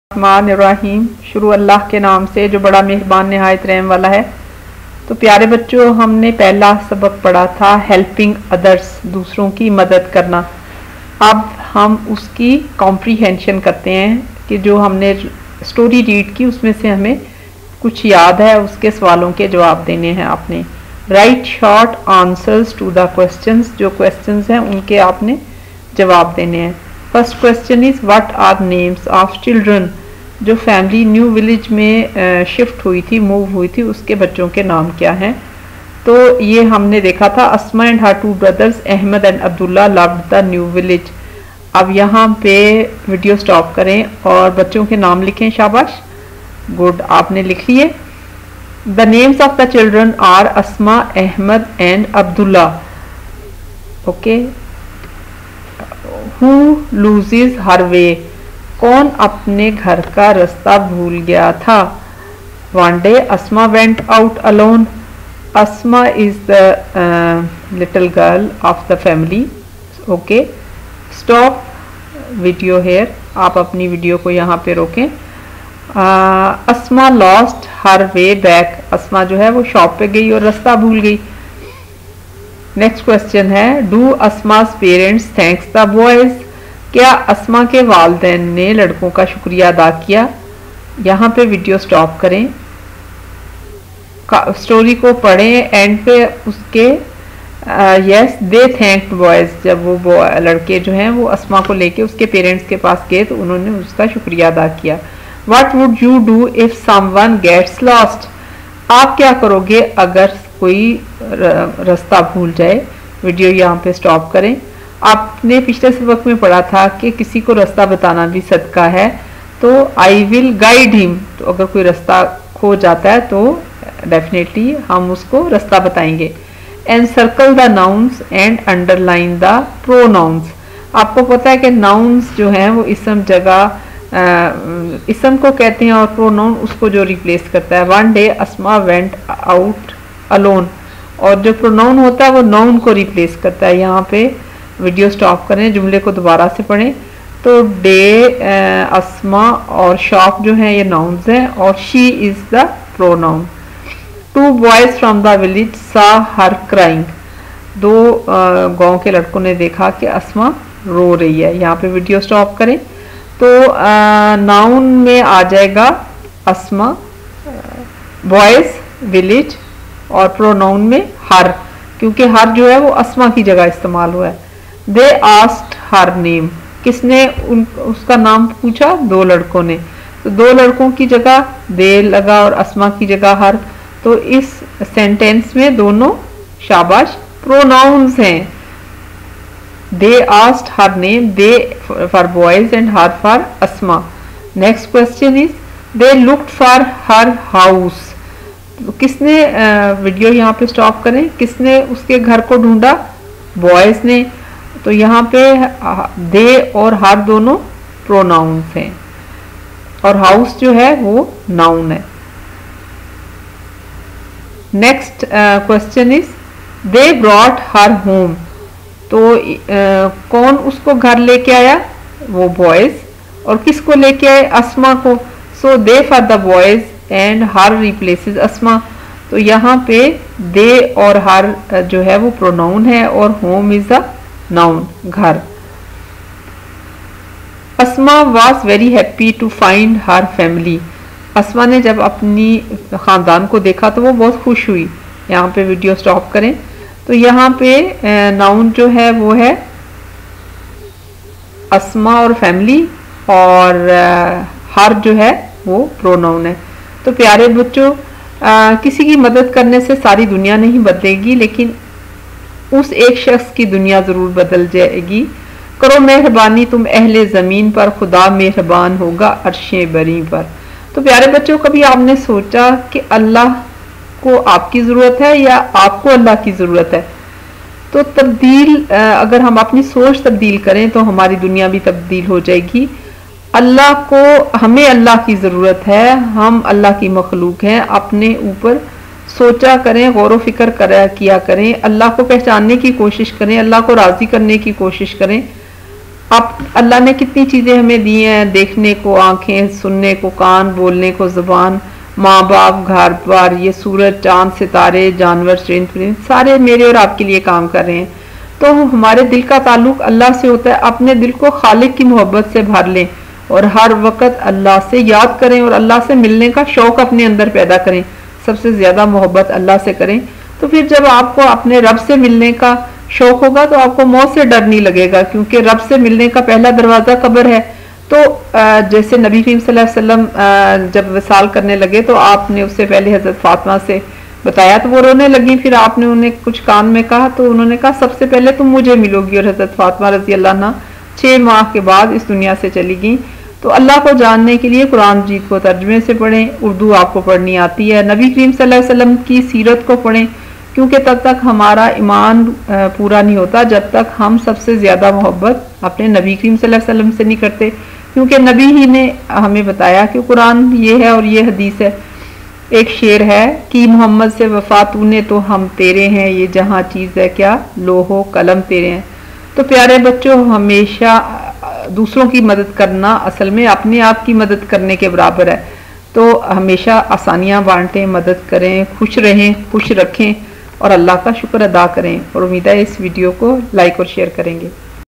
Helping others/English in Urdu
Class 3 English L 41, comprehension story helping others in Urdu
comprehension of story helping others  for grade 3 kids in urdu